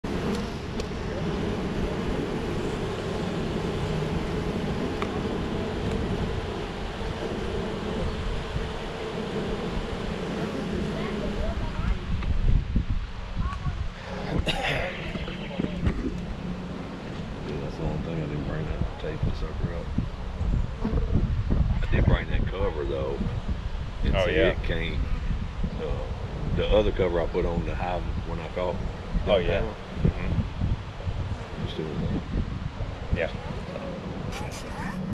Kolb Swarm Nashville-Donelson April 2019Part 3 of 4
Swarm in tree finally in the box